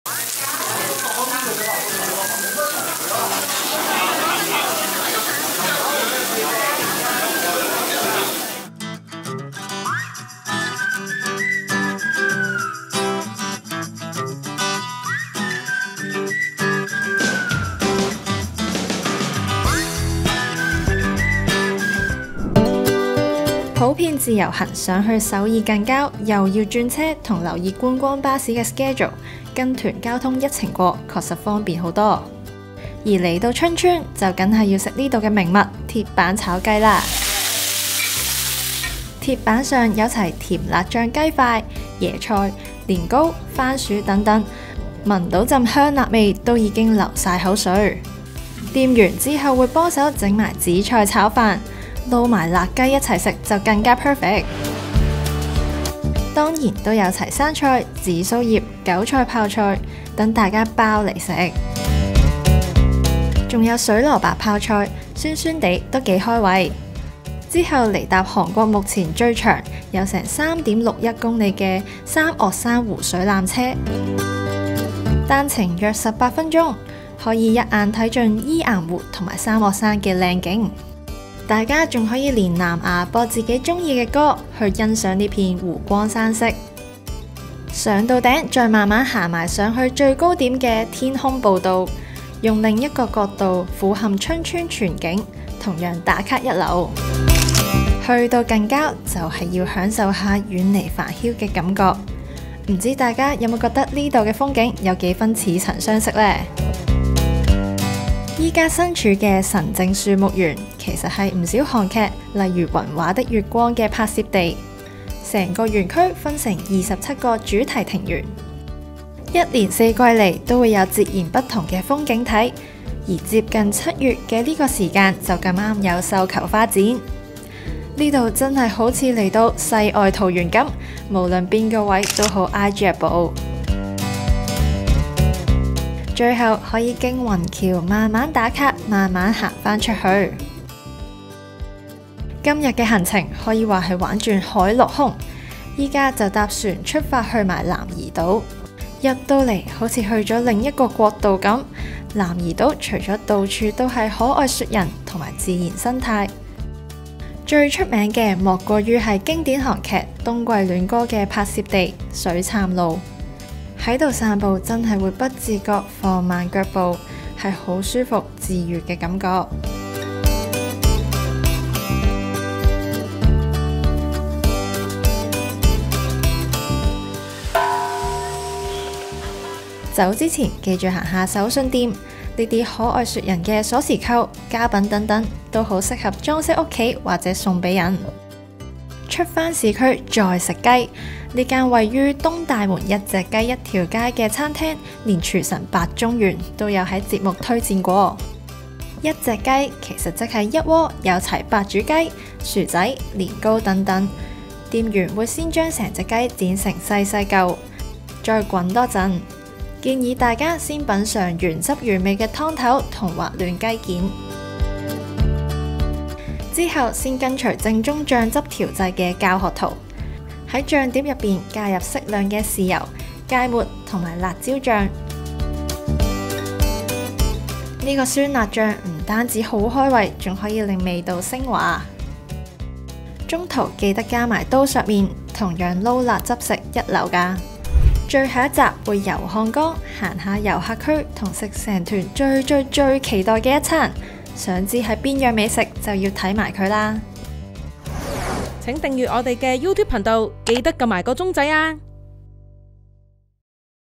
實試跟團貼地遊 | 海陸空玩轉首爾近郊 名廚白種元推薦餐廳！ 三岳山湖水纜車/南怡島/晨靜樹木園
普遍自由行想去首爾近郊，又要轉車，又要留意觀光巴士時間表等等，跟團交通一程過確實方便不少！加上旅行團已經安排好三岳山湖水纜車、南怡島、晨靜樹木園的門票，大家就更可以零煩惱地沉醉於迷人大自然之中。今集還會吃勻兩款雞料理，立即去片揭曉▶️！

影片內介紹的美食checkpoint：

📍春川炒雞一條街 必試惹味春川鐵板炒雞

來到春川，除了南怡島、晨靜樹木園等等著名景點，就必定要品嘗當地名物——鐵板炒雞！鐵板上有齊甜辣醬雞塊、椰菜、洋蔥等等材料，炒的時候香氣撲鼻，已令人垂涎欲滴。因應香港人口味，旅行團特意讓店家特製減辣版，但味道依然香辣惹味，配搭紫菜炒飯更是大滿足。

📍東大門一隻雞一條街 嫩滑白煮雞配搭秘密醬汁

東大門不只出名購物，其一隻雞一條街也吸引不少遊客慕名而來。「一隻雞」其實即是白煮雞，店員會先剪成小塊再滾多一會兒。當鍋中的年糕浮面約5分鐘後，建議大家先品嘗原汁原味的湯頭和滑嫩雞件。隨後再跟隨正宗醬汁調製教學圖，在醬碟裡加入適量醬油、芥末、辣椒醬。酸辣醬汁非常開胃，還令味道昇華。中途緊記加入刀削麵，同樣拌辣汁吃一流。

鳴謝：永安旅遊

🔔錯過左之前個兩集？呢到有得睇返啦！

bmgproductionmusic